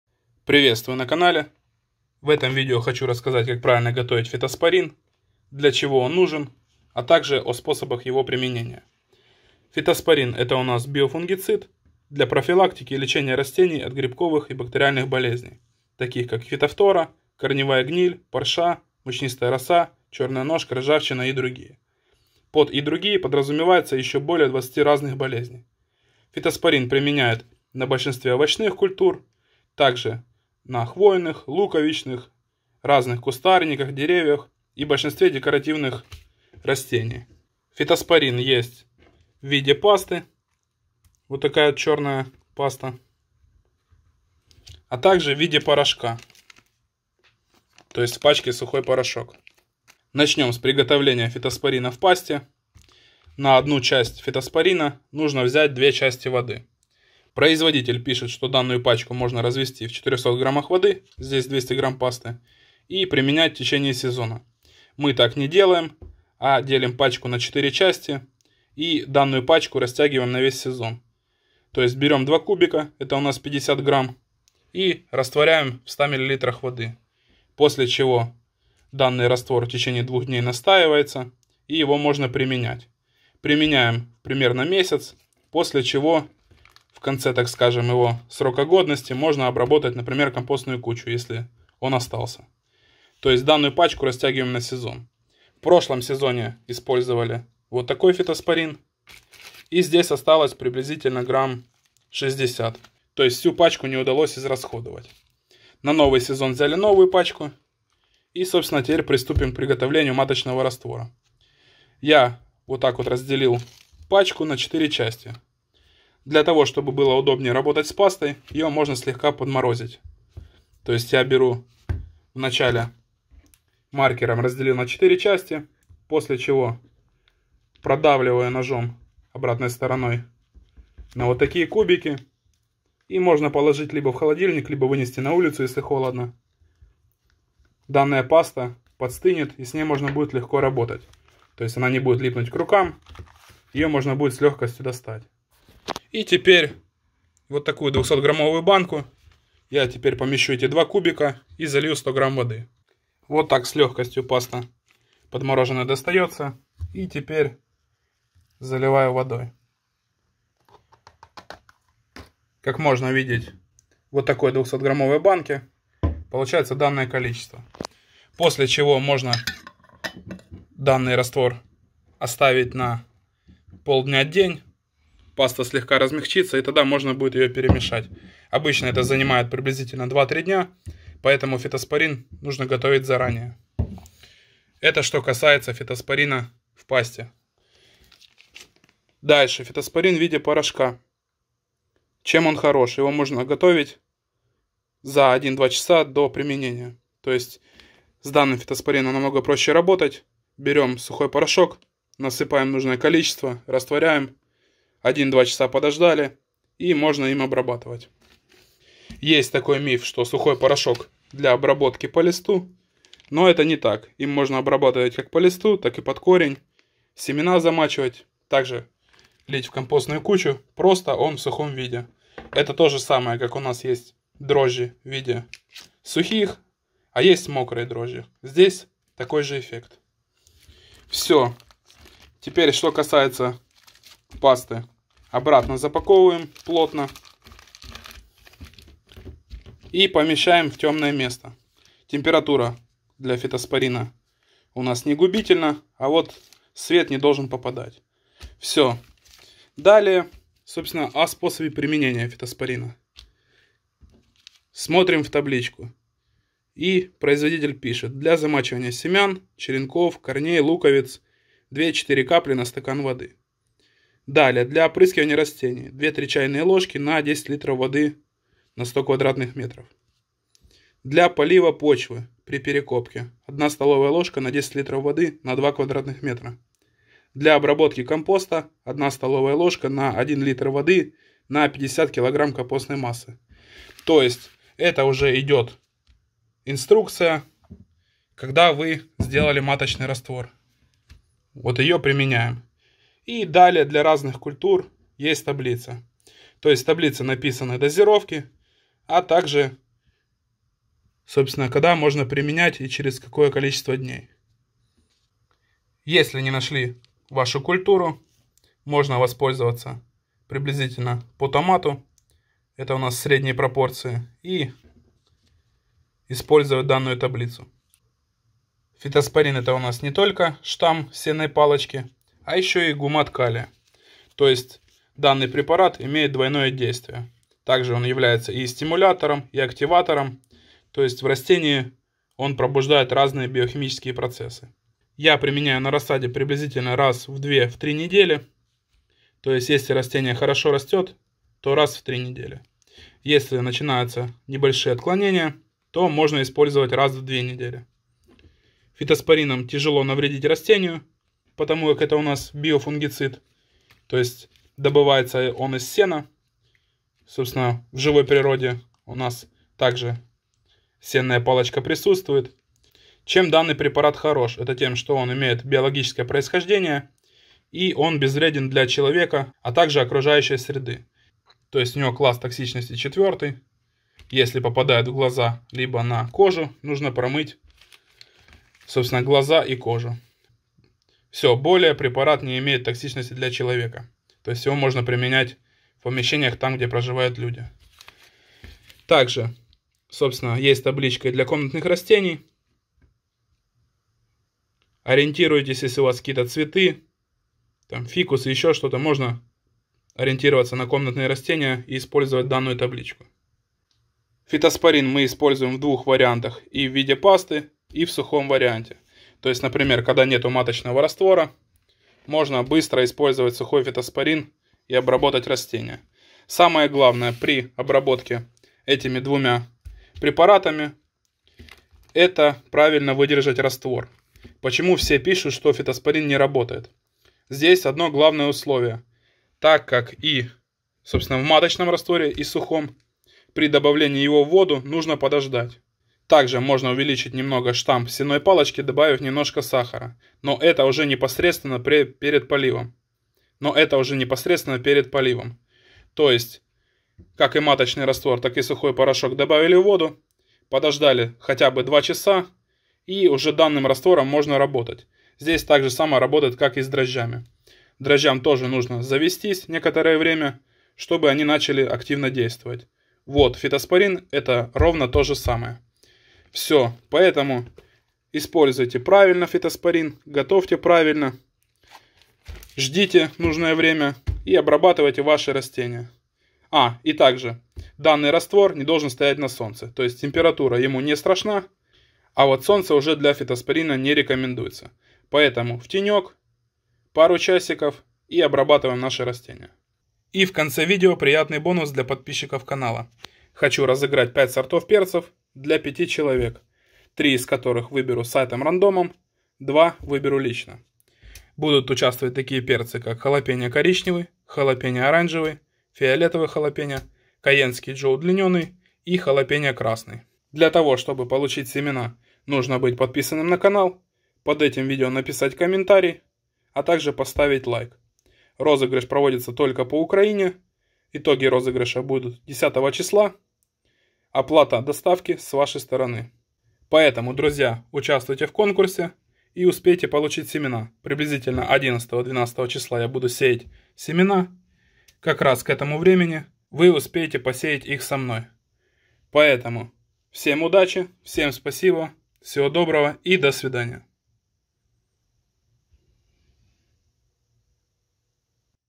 Как правильно приготовить средство от болезней + бонус в конце.
Как правильно приготовить фитоспорин + бонус в конце.

Фитоспорин - это биологический препарат, который используется для защиты растений от болезней. Обычно он доступен в виде порошка или концентрата для растворения. Вот общие шаги по его приготовлению и использованию:

Подготовка раствора:
В соответствии с инструкциями на упаковке измерьте нужное количество фитоспорина.
Растворите его в необходимом количестве воды. Обычно это 1 часть фитоспорина и 2 части воды, следуйте инструкциям на упаковке.

Перемешивание:
Тщательно перемешайте раствор, чтобы фитоспорин равномерно распределился в воде.

Нанесение:
Распылите раствор фитоспорина на листья и стебли растений, которые нужно защитить.
Проводите обработку в тихую погоду, предпочтительно вечером или в ранние утренние часы, чтобы избежать высыхания раствора под воздействием солнца.

Повторение обработки:
Для наилучших результатов повторяйте обработку в соответствии с рекомендациями на упаковке или советами специалистов.

Безопасность:
Приготовленный раствор фитоспорина следует использовать в соответствии с инструкциями производителя.
Не допускайте попадания раствора на кожу или в глаза. Если это случится, тщательно промойте область водой.
Работайте в перчатках и защитной маске, чтобы избежать контакта с препаратом.

Так же по всем вопросам пишите в директ:
Всего доброго и до встречи в новых видео.
Удачи!